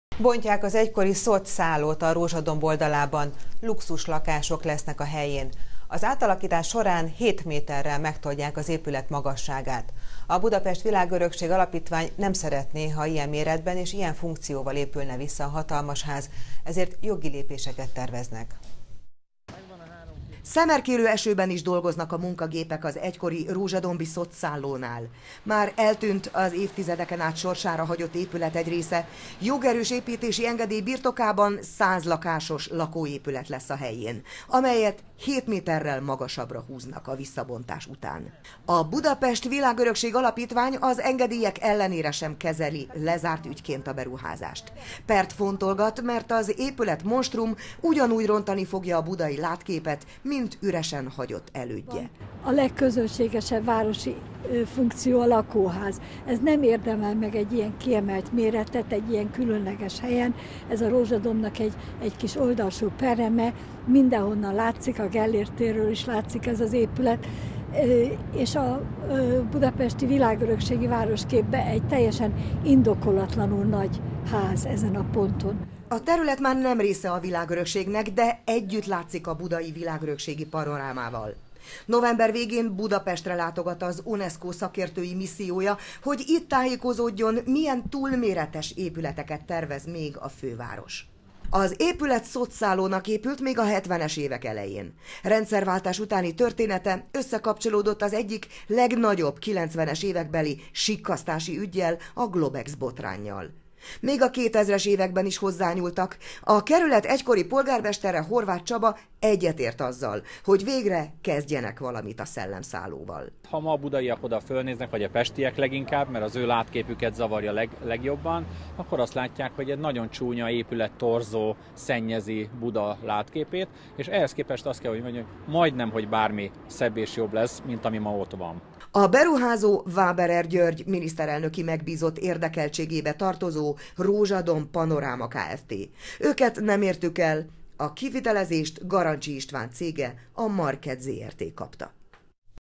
Még nagyobb monstrum épül a budai szellemszálló helyén
Bontják az egykori SZOT-szállót a Rózsadomb oldalában. Luxuslakások lesznek a helyén, és 7 méterrel megtoldják az épület magasságát. A Budapest Világörökség Alapítvány nem szeretné, ha ilyen méretben és ilyen funkcióval épülne vissza a hatalmas ház. Jogi lépéseket fontolgatnak.